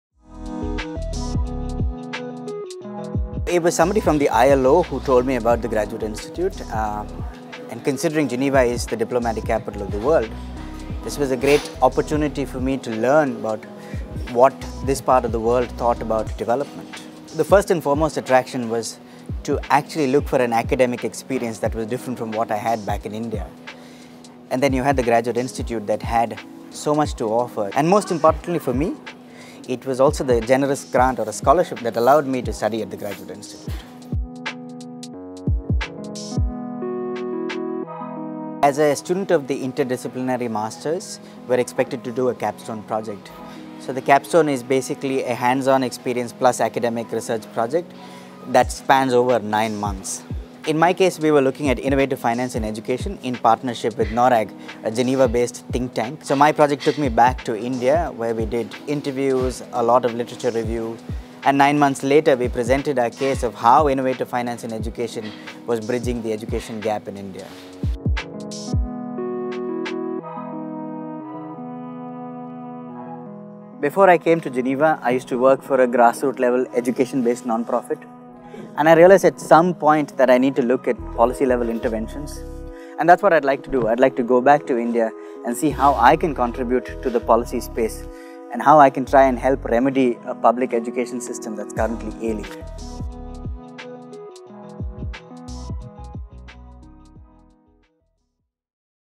Studying to Improve the Future of Education in India
Karun Gopinath is pursuing a Master in Development Studies. As part of his programme's required Capstone – a nine-month group research project where students partner with organisations in International Geneva – he and his team looked into innovative ways to bridge the education gap in India, a project close to his heart.